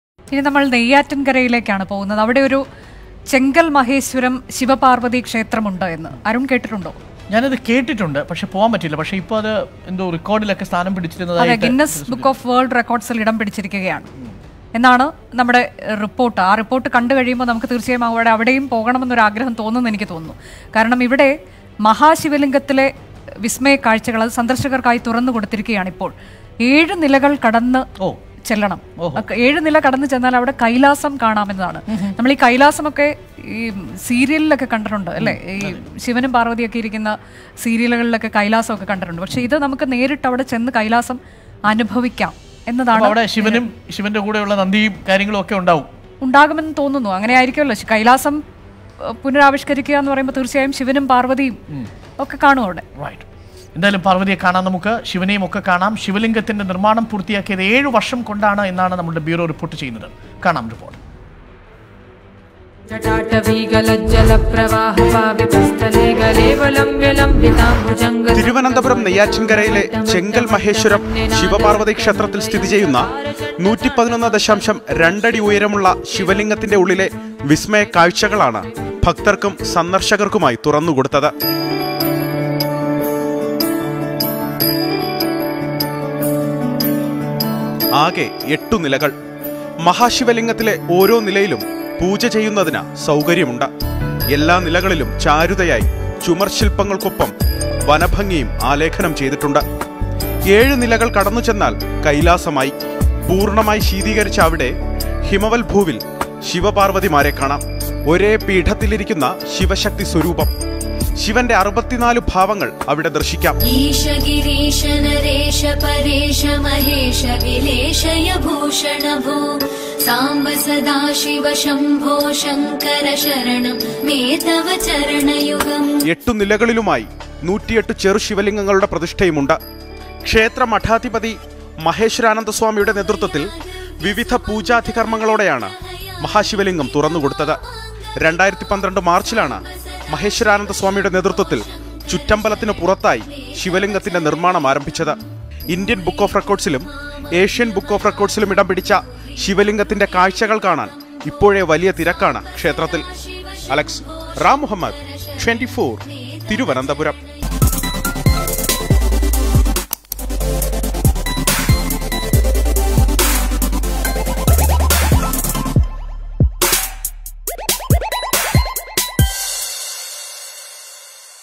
Chenkal maheshwram Shiva Parvati kshethram news on 24news
Biggest shiva lingam in the world